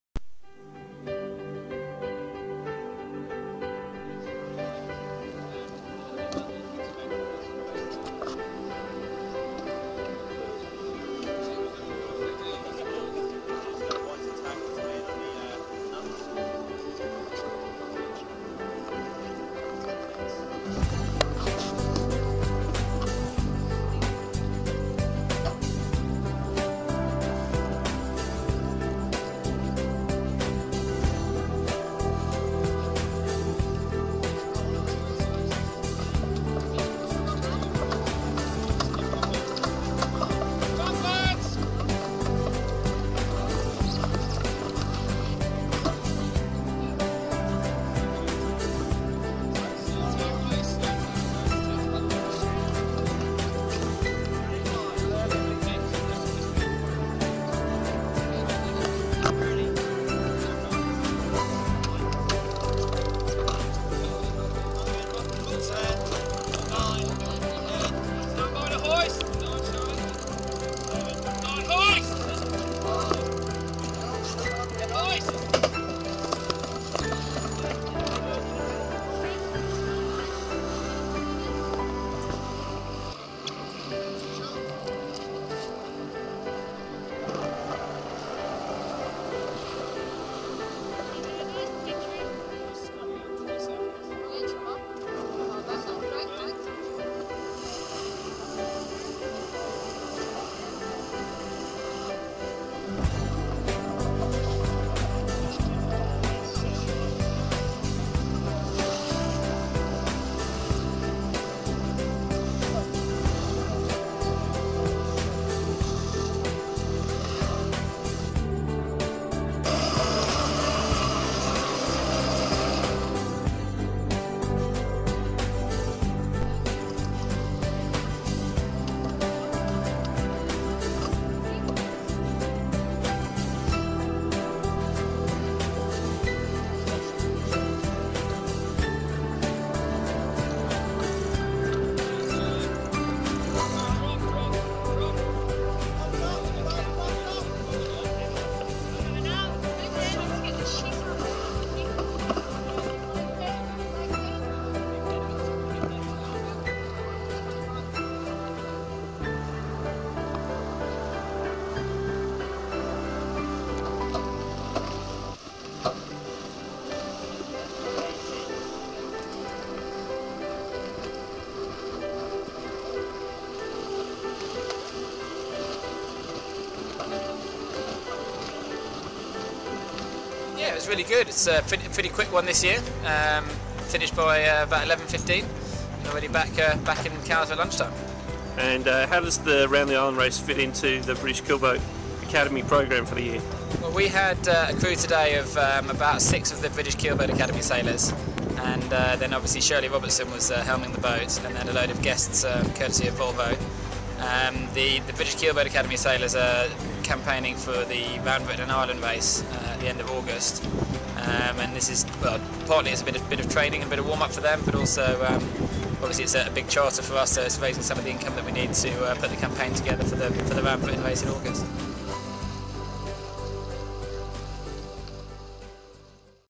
Round the Island Race 2010 - Aboard Volvo Team For Life TP52
- 2010 J P Morgan Asset Management Round the Island Race: Includes pre-start footage with Shirley Robertson on the helm and some amazing scenes of sunrise and the J Class yacht Vashelda. Also features IDEC trimaran at the needles and Ecover Extreme 40. Racing near the end against BOB - Farr 52 owned by BP CEO Tony Hayward